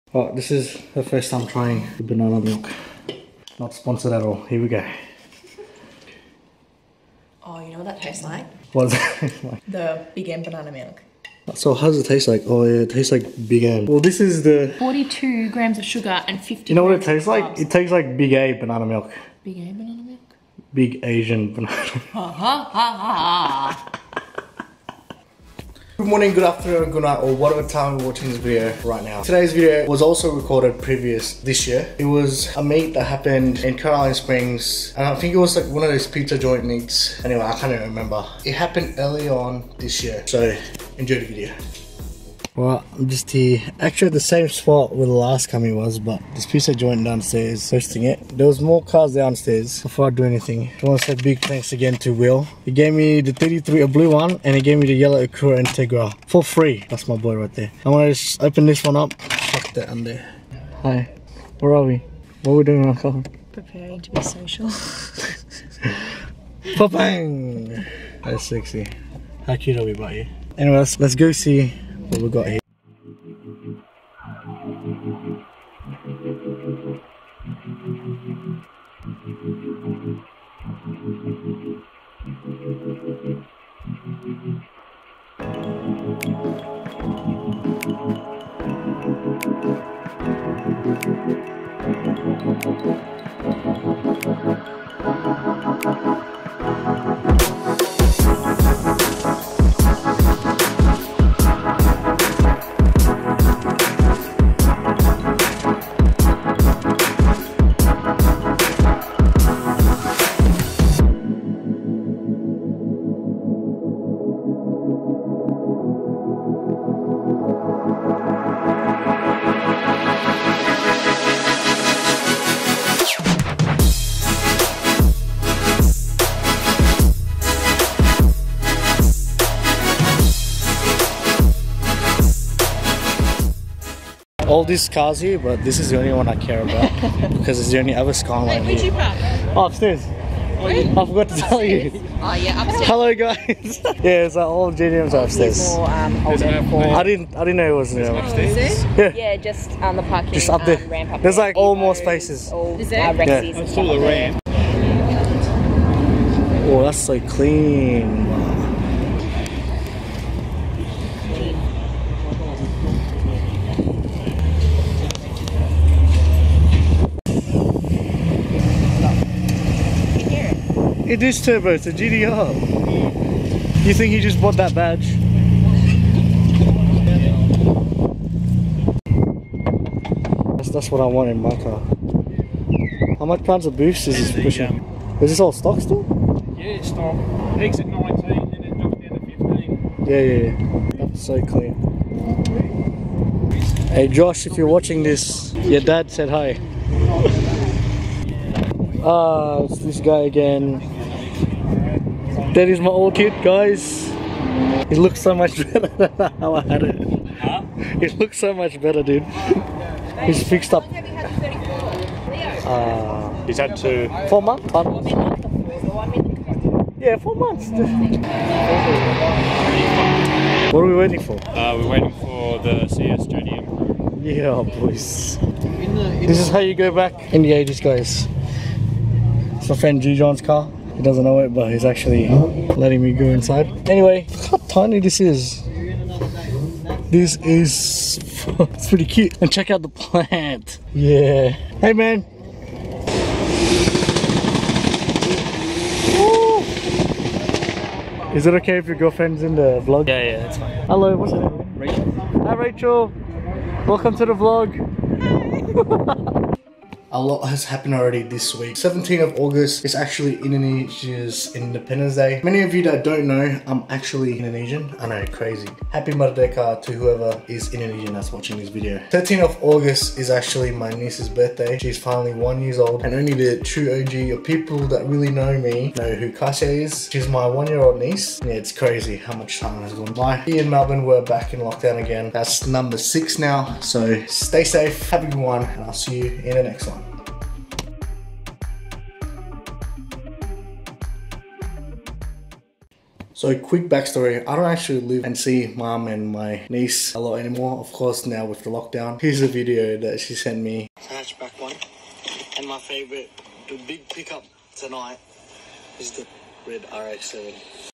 The Car Meet That I Forgot About
Caroline Springs Car Meet
Occured earlier on this year!

Miss going to a meet
Melbourne is back into another lockdown
Number 6
Stay safe!

Big Happy Birthday to my Niece Cassia
Finally turned 1 year old!

Selamat Hari Merdeka to us Indonesians!

Click that sub button if you haven't already